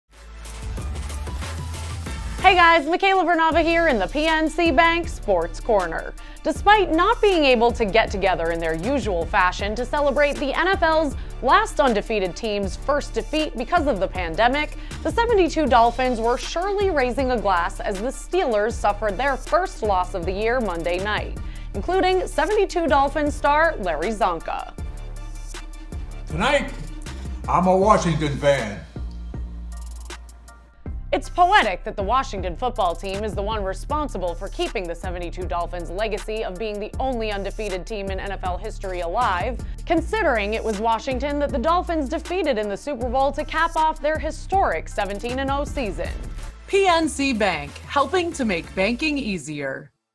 1972 Dolphins Fullback, Larry Csonka, Celebrates Steelers' Defeat
With the Pittsburgh Steelers' loss, the 1972 Miami Dolphins still have the only perfect season in NFL history, and that means it's time to celebrate.